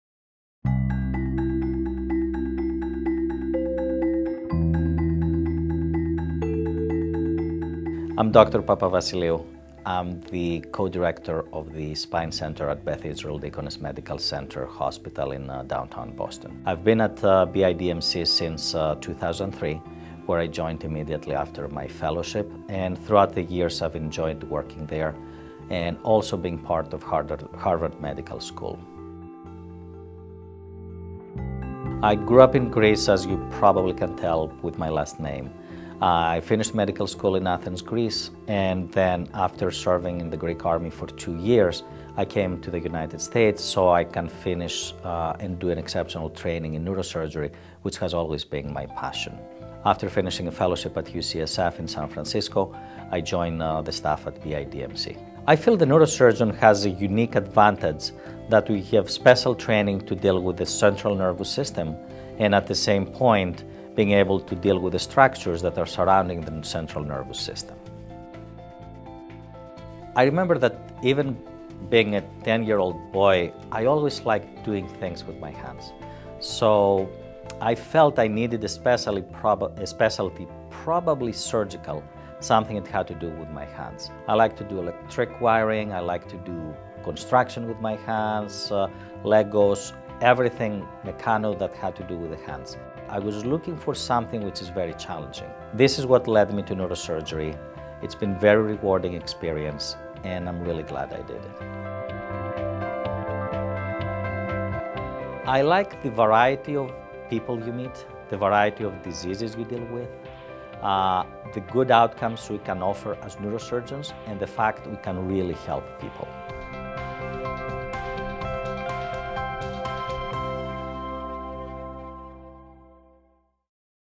Meet Efstathios Papavassiliou, MD, of BID-Milton's Spine Program
Get to know Beth Israel Deaconess Hospital-Milton's Spine Program Neurosurgeon Efstathios Papavassiliou, MD.